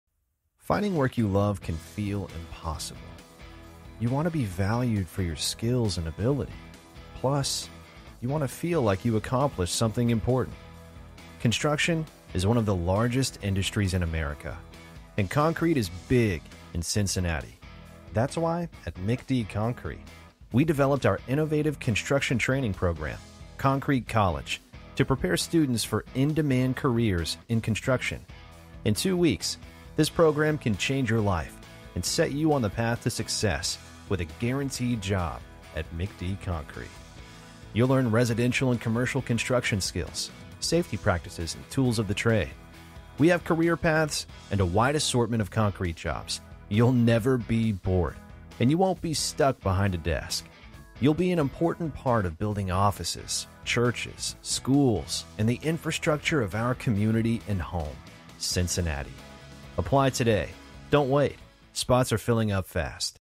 Video Marketing 1 Minute Overview - What Is Concrete College
A large construction firm in Cincinnati needed a complete marketing campaign to gain awareness and enrollment for their construction apprentice program. BigOrange held a strategy and StoryBrand discovery session to clarify messaging. We built a landing page, video overview and complete campaign with Google, Facebook and TikTok ads in expedited tight turnaround of 10 Days! The results included 55 applicants in 3 weeks.